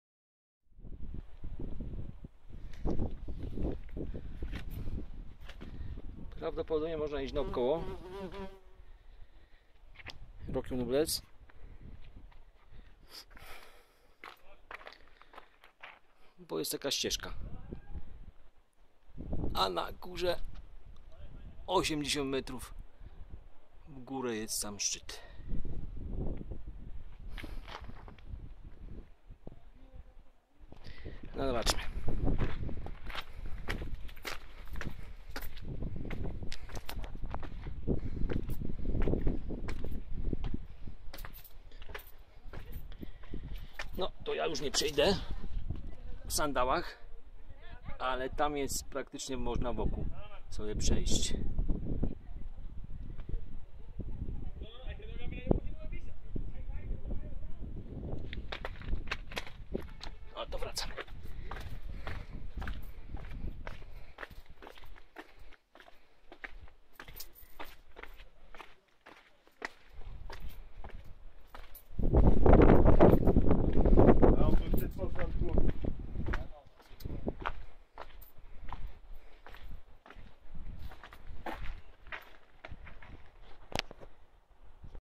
Gran Canaria - Roque Nublo - wokol skaly
Drugi najwyższy szczyt Gran Canarii. Wysokość 1813 mnpm. Swięta góra dla miejscowych aborygenów - Guanczów. Wejście łagodne, można spokojnie pójść w sandałach. Na sam szczyt bez wyposażenia wspinaczkowego nie polecam :) Parking nie jest duży, dlatego warto podjechać tam rano.